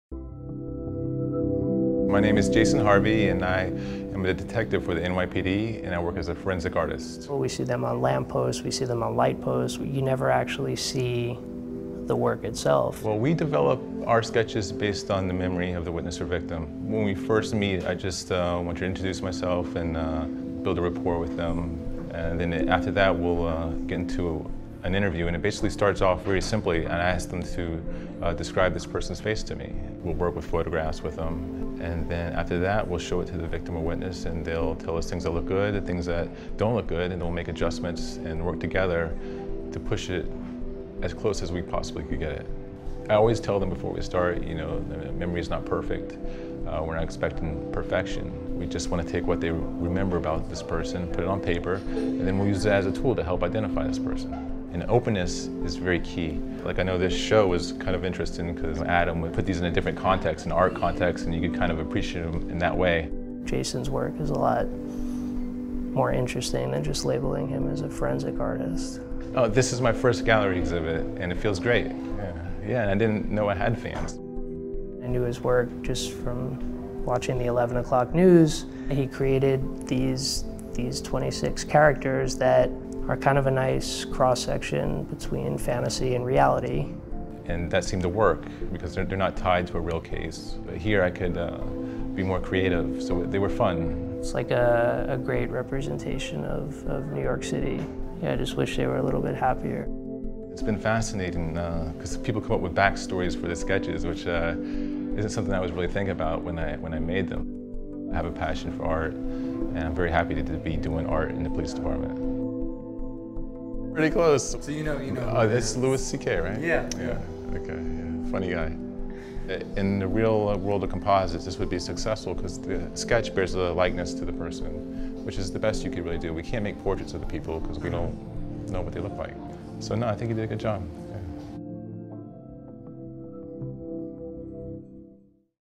Watch NYPD's Incredible Sketch Artist Draw Louis C.K.
We described Louis C.K. to police sketch artist and Detective Jason Harvey, a 16-year veteran of the NYPD. We didn't tell him who we were describing—here's what he came up with.